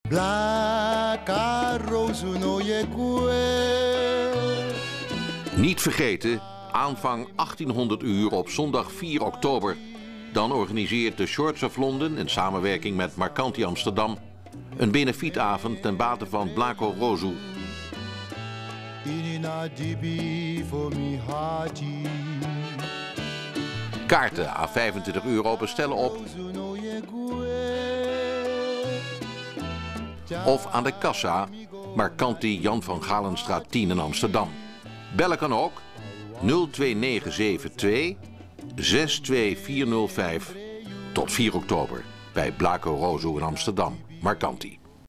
Benefietavond Blaka rosoe 4oktober amsterdam marcanti
4 Oktober presenteert shorts of london een Benefietavond t.b.v  Blaka Rosoe in Amsterdam Marcanti.